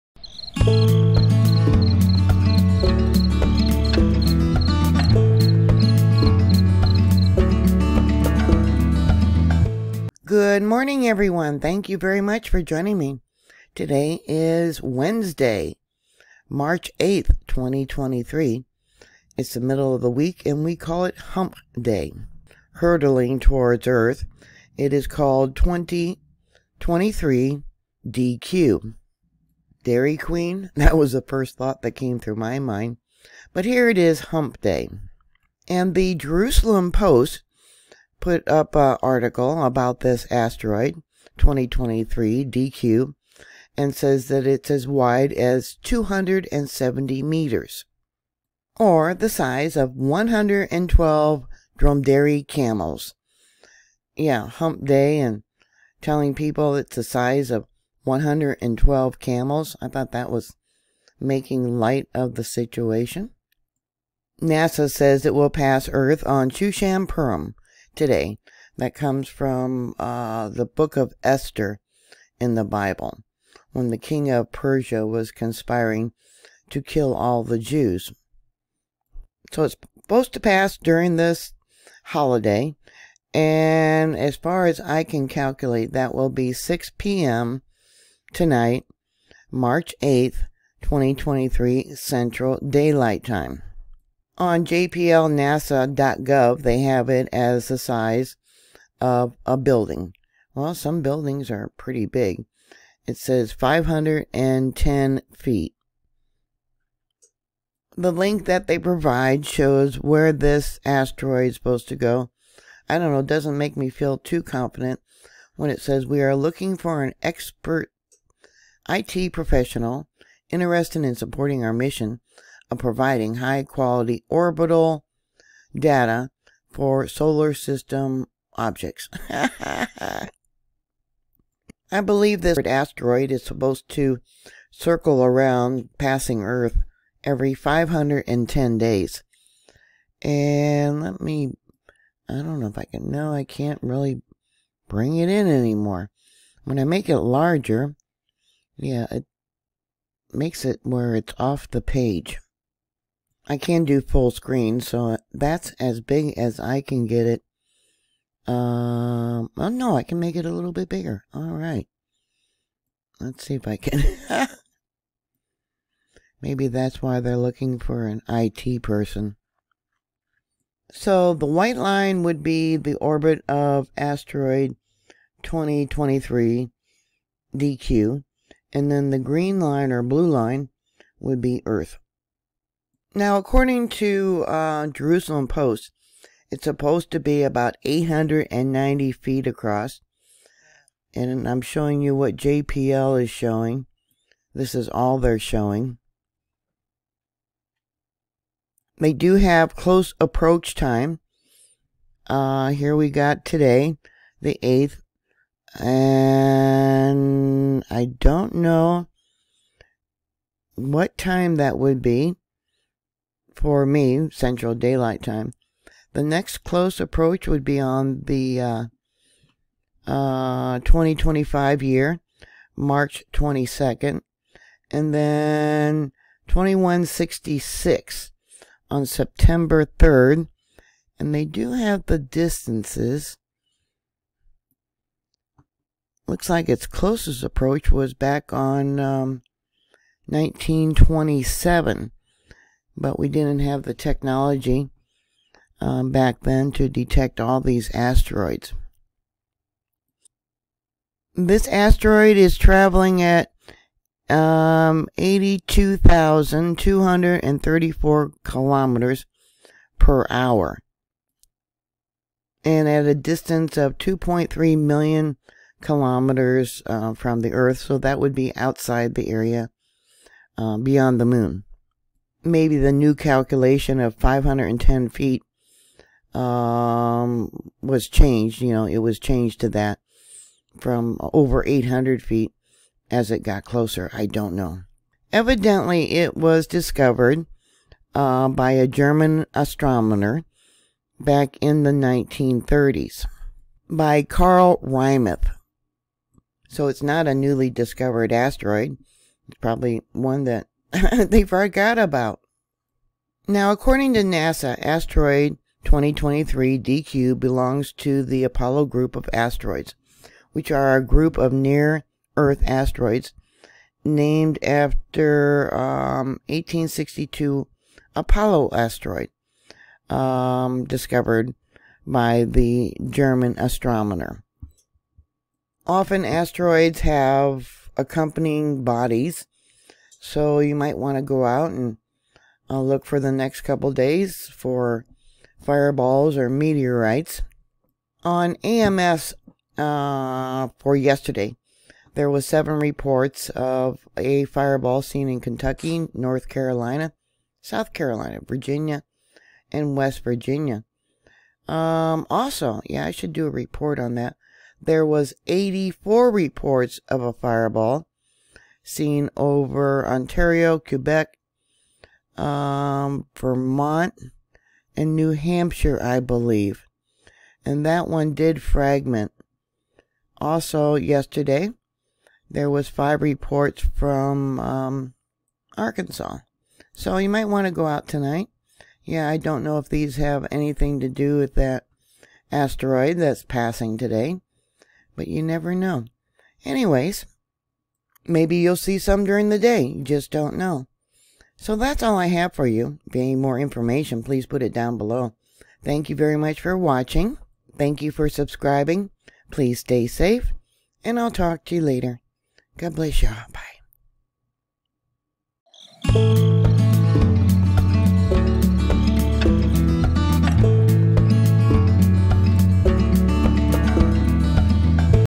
🌠Asteroid 2023 DQ Close Approach Today Over 500 FT Across 🚀🌑
Asteroid 2023 DQ will make a close approach today. March 8, 2023.